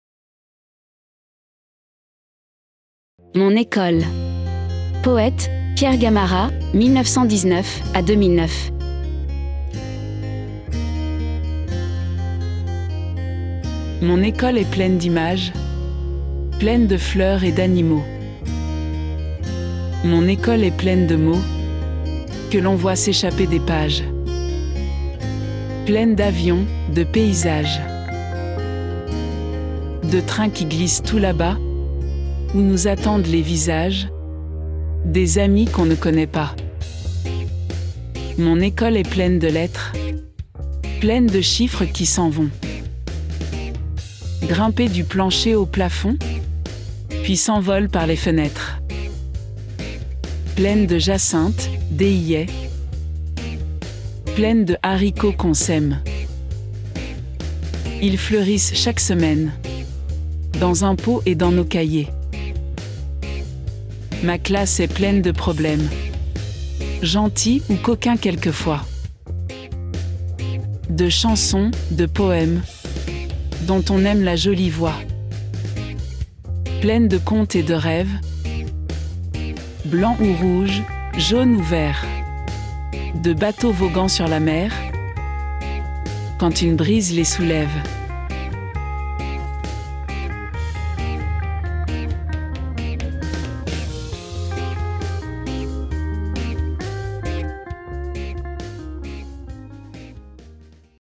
Mon école I Pierre GAMARRA
Mon école est pleine d'images,
Pleine de fleurs et d'animaux,
Mon école est pleine de mots
Que l'on voit s'échapper des pages,
Pleine d'avions, de paysages,
De trains qui glissent tout là-bas
Où nous attendent les visages
Des amis qu'on ne connaît pas.

Mon école est pleine de lettres,
Pleine de chiffres qui s'en vont
Grimper du plancher au plafond
Puis s'envolent par les fenêtres,
Pleine de jacinthes, d'œillets,
Pleine de haricots qu'on sème ;
Ils fleurissent chaque semaine
Dans un pot et dans nos cahiers.

Ma classe est pleine de problèmes
Gentils ou coquins quelquefois,
De chansons, de poèmes,
Dont on aime la jolie voix
Pleine de contes et de rêves,
Blancs ou rouges, jaunes ou verts,
De bateaux voguant sur la mer
Quand une brise les soulève.